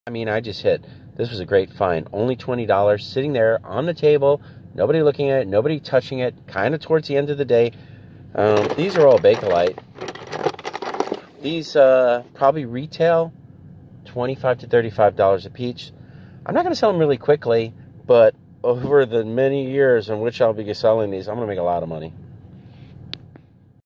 Me scoring big time on some Bakelite Scotty Dogs! My best garage sale finds.
S
M